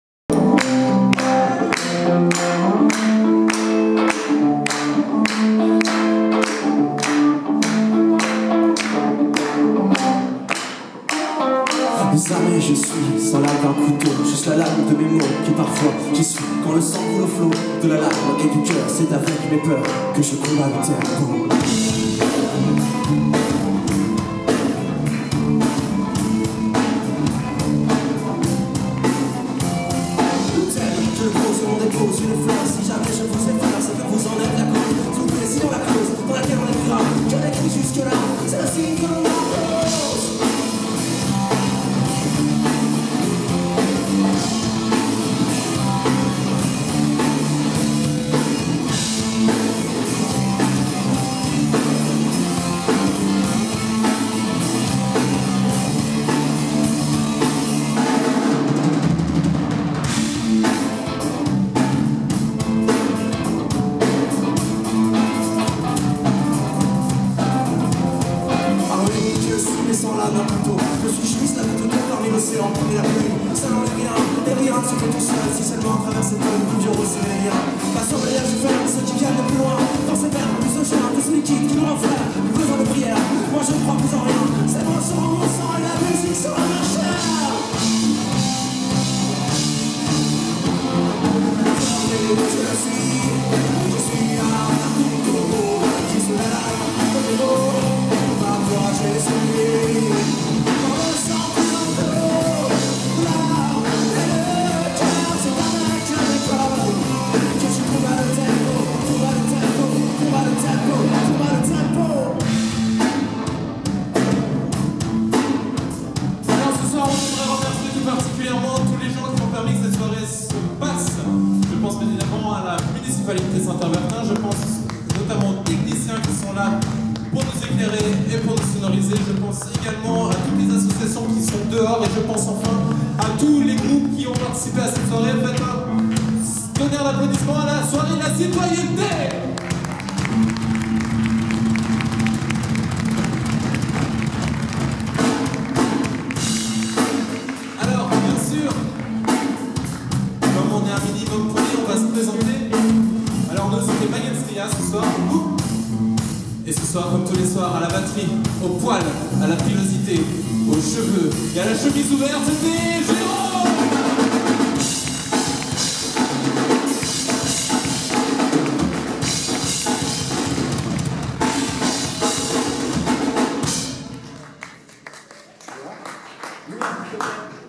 Magenstria le 06/03/15 - Nuit de la citoyenneté au Nouvel Atrium à Saint-Avertin
Organisée par la commune de Saint-Avertin et Fred Fouin !!!